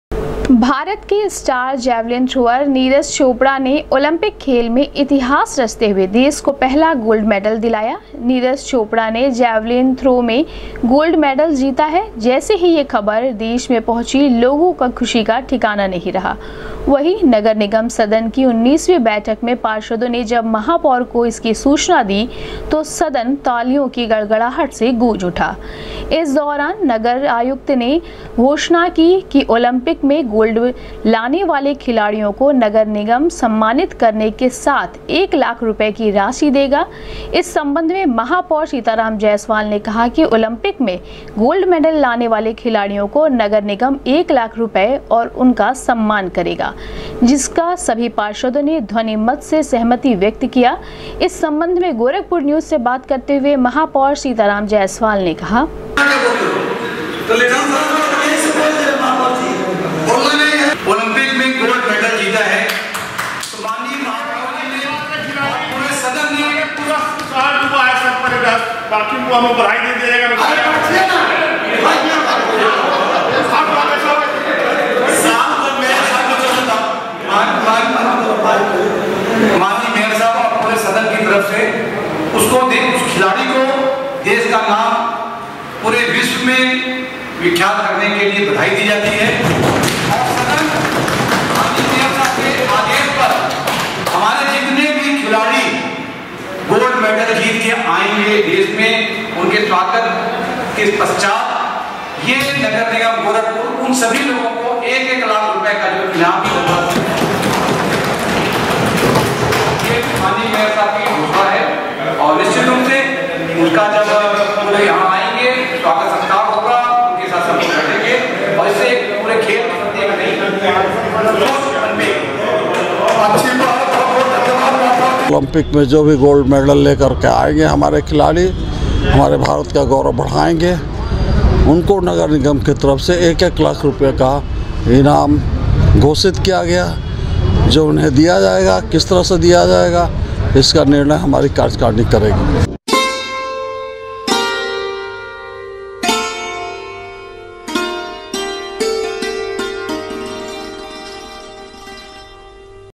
भारत के स्टार जैवलिन थ्रोअर नीरज चोपड़ा के ओलंपिक खेल मे गोल्ड मेडल जीतने पर नगर निगम सदन ने ध्वनि मत
भारत के स्टार जैवलिन थ्रोअर नीरज चोपड़ा के ओलंपिक खेल मे गोल्ड मेडल जीतने पर नगर निगम सदन ने ध्वनि मत से दी बधाई, गोल्ड मेडल पाने वाले खिलाड़ियों को नगर निगम एक लाख रुपए देकर करेगा सम्मानित